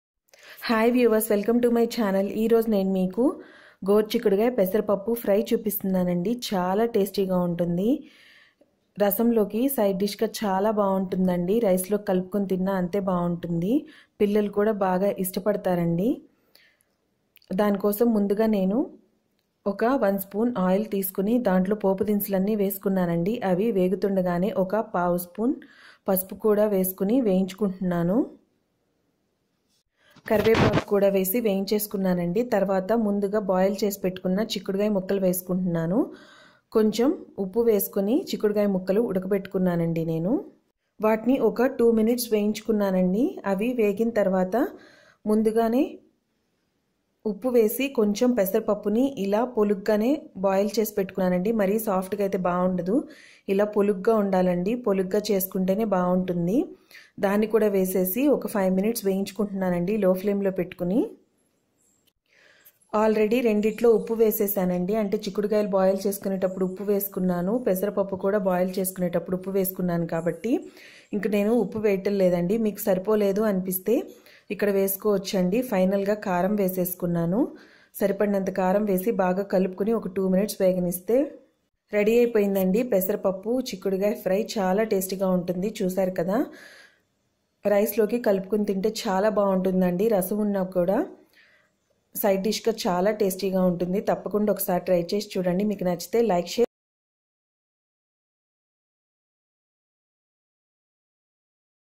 Goruchikkudu pasara poppu fry recipe in Telugu//simple lifestyle my Telugu channel
Hi friends
Good morning
Today vedio
Goruchikkudu pasara poppu fry